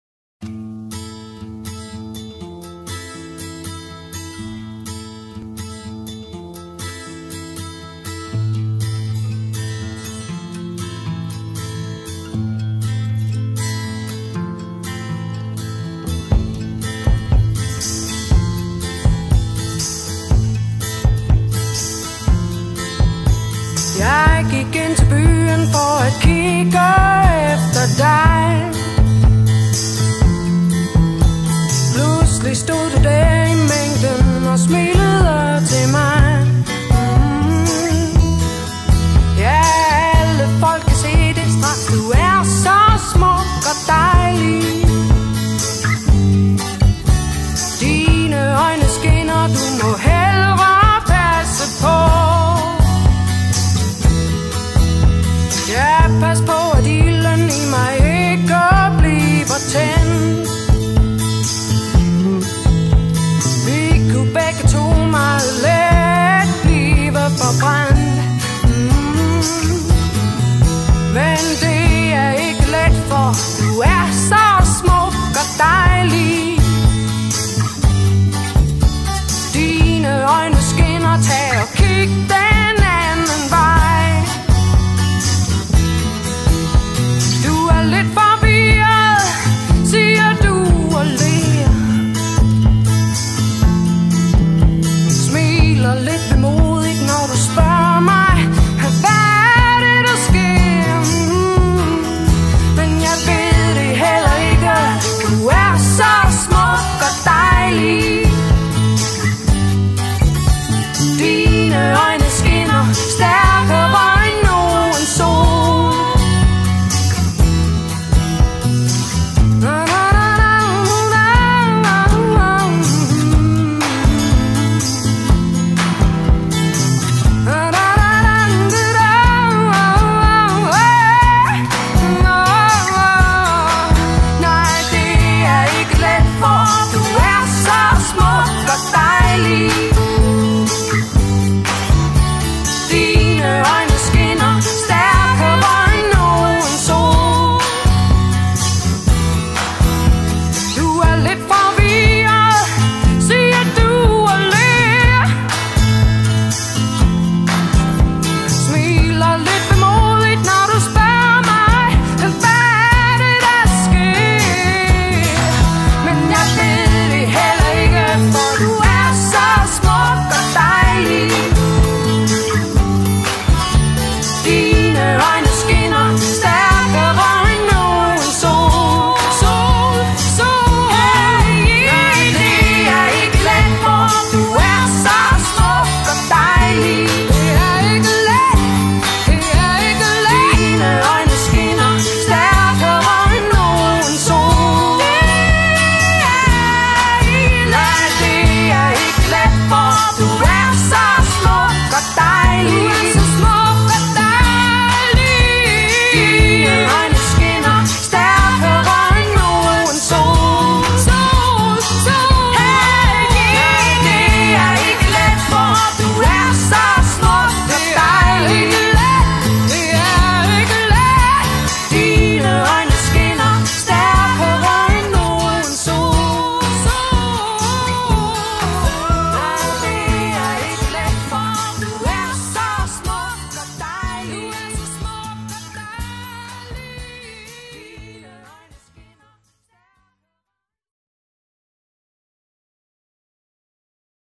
Til den du elsker
Hvis du modtager link til denne video, er det fordi at der er en som elsker dig højere end noget andet.

Musik af Anne Linnet